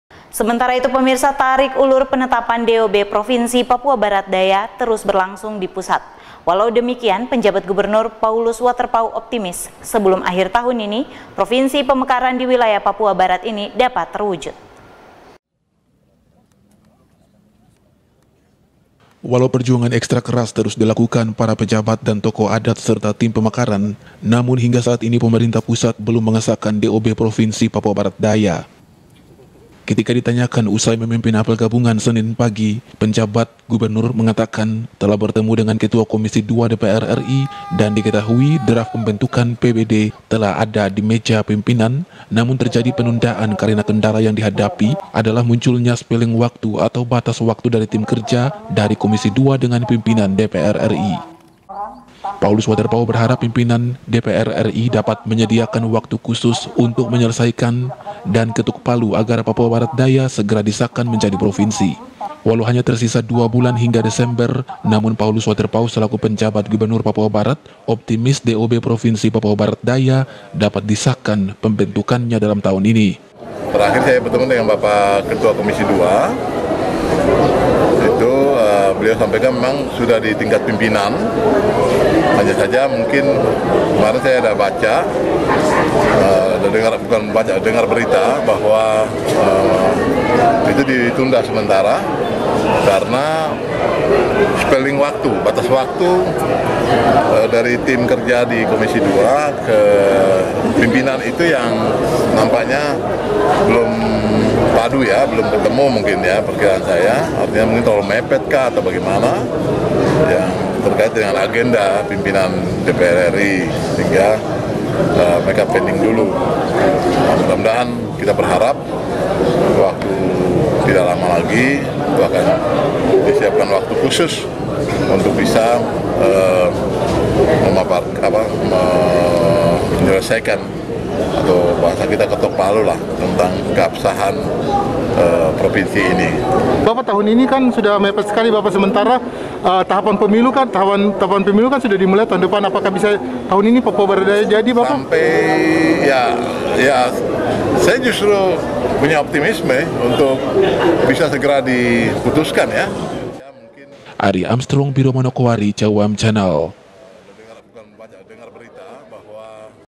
TARIK ULUR PENETAPAN PROVINSI PBD TERUS BERLANGSUNG
TARIK ULUR PENETAPAN DOB PROVINSI PAPUA BARAT DAYA TERUS BERLANGSUNG DI PUSAT WALAU DEMIKIAN PENJABAT GUBERNUR PAULUS WATERPAUW OPTIMIS SEBELUM AKHIR TAHUN INI PROVINSI PEMEKARAN DI WILAYAH PAPUA BARAT INI DAPAT TERWUJUD